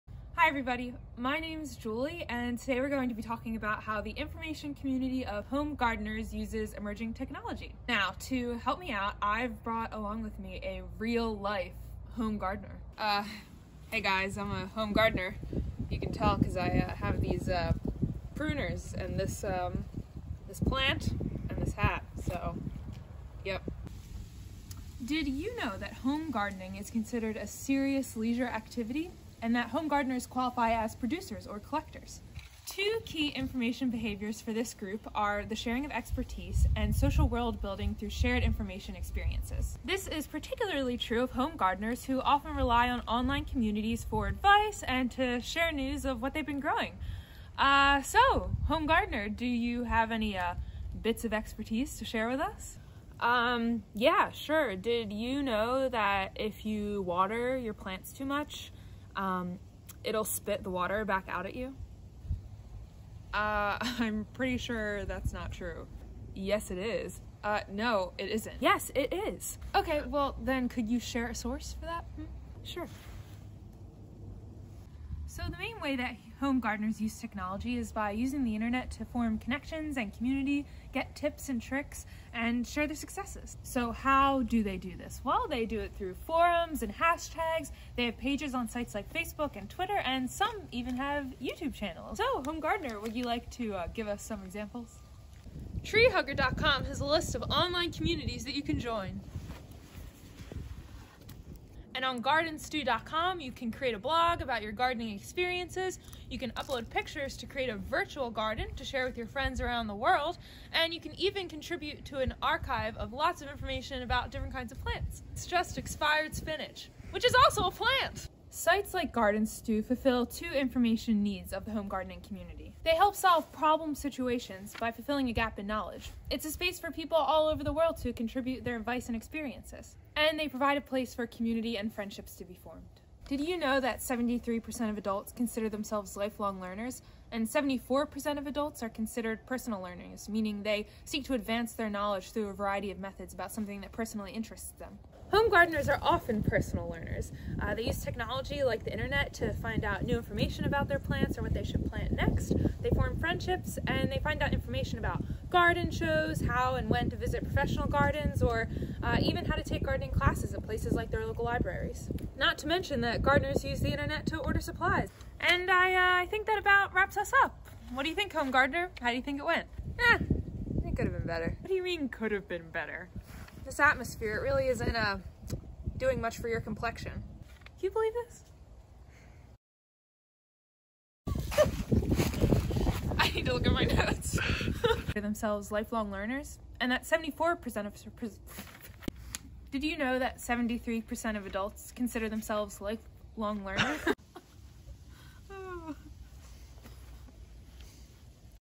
INFO 200 Blog #7
Julie Peterson INFO 200
MLIS first semester

References:

Frank (GardenStew Founder). (2006, July 12). GardenStew starter pack/about GardenStew. GardenStew.

Gonzalez, R. (2018, October 11). 10 online gardening communities you should join. Treehugger.

Horrigan, J. B. (2016, March 22). Lifelong learning and technology. Pew Research Center Internet and Technology.

King, A. (2019, December 27). Do cheese plants cry? (Monstera Deliciosa). Indoor Home Garden.

Mansourian, Y. (2019, December 1). How passionate people seek and share various forms of information in their serious leisure. Journal of the Australian Library and Information Association. 69(1), 17–30.

O’Brien, H., & Greyson, D. (2018, March 1). Diverse information needs. In S. Hirsh (Ed.), Information services today: An introduction (2nd ed., pp. 40–51). Rowman & Littlefield Publishers.